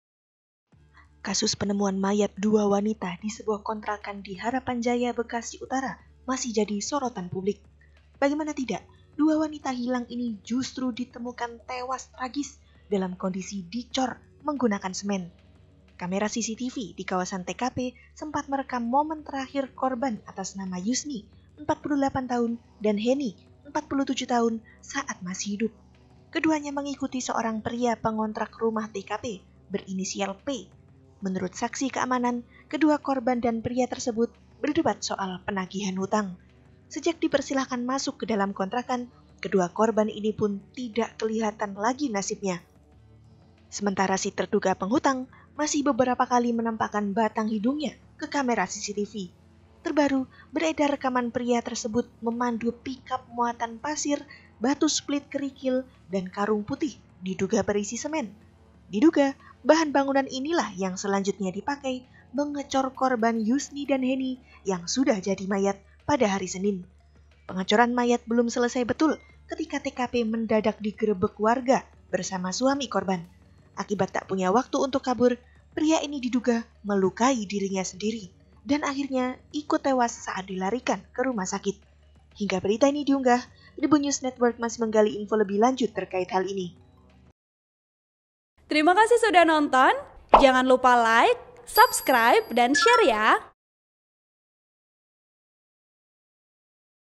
DETIK-DETIK PEMBUNUH MAYAT DICOR Beli Kerikil Bangunan, Diduga Dua Wanita Bekasi Sudah Tewas
TRIBUNJATIM.COM - Kasus penemuan mayat dua wanita Bekasi di sebuah kontrakan di Harapan Jaya, masih jadi sorotan publik.

Bagaimana tidak, dua wanita hilang ini justru ditemukan tewas dalam kondisi dicor menggunakan semen.

Sejak dipersilahkan masuk ke dalam kontrakan, kedua korban ini pun tidak kelihatan lagi nasibnya.

Keesokan harinya, terduga pembunuh keluar untuk menyambut pick up yang mengirim paket batu split kerikil bangunan, pasir, dan karung diduga isi semen.

Diduga kuat, pelaku digerebek warga dan suami korban saat pekerjaan cor mayat belum sempurna

sEMENTARA